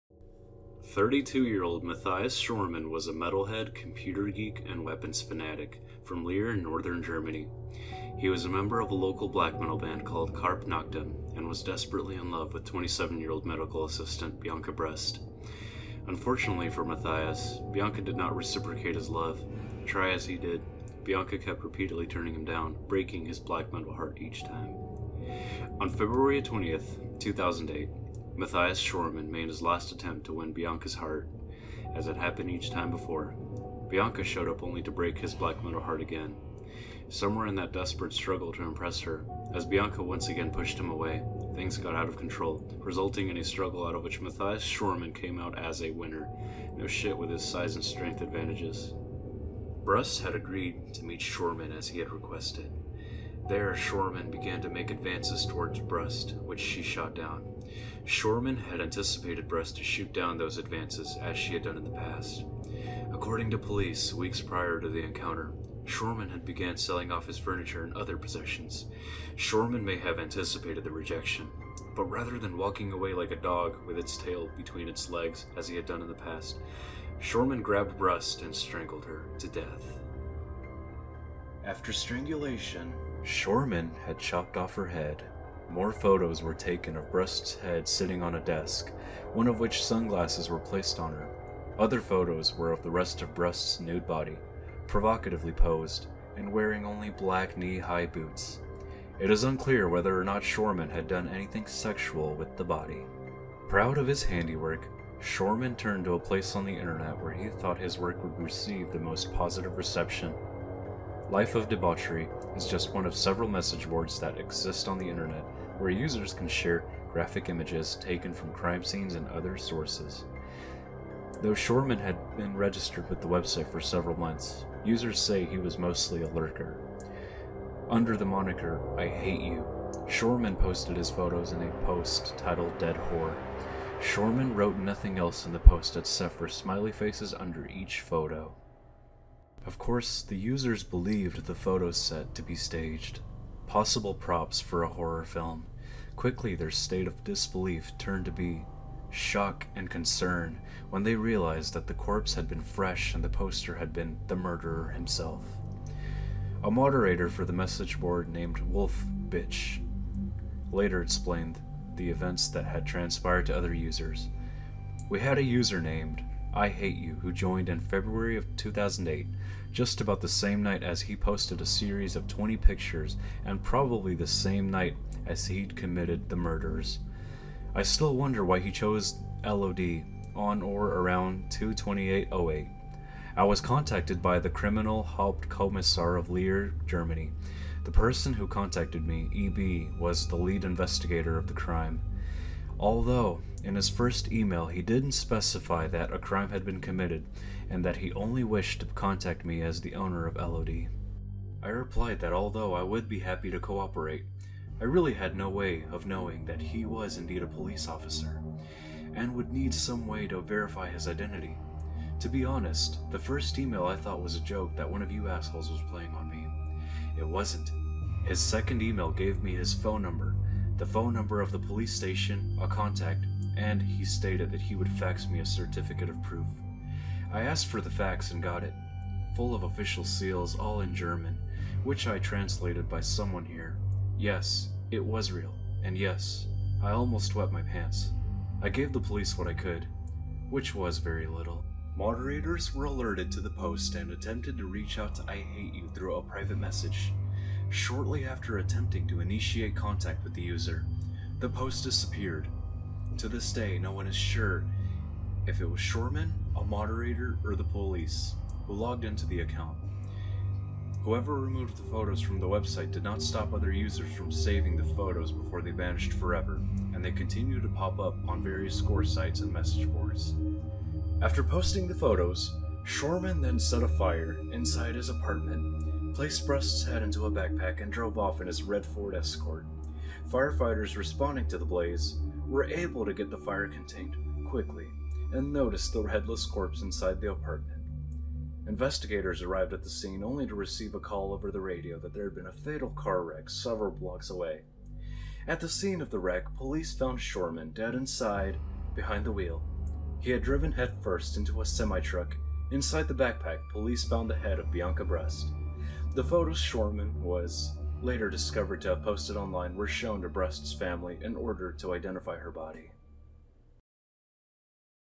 The disturbing case of Matthias Schoorman
This is the disturbing story of a man who was a black metal guitarist for a black metal band known as Carpe Noctem. Dive deep into the video to discover the gruesome details of murdering 27-year-old Bianca Brust.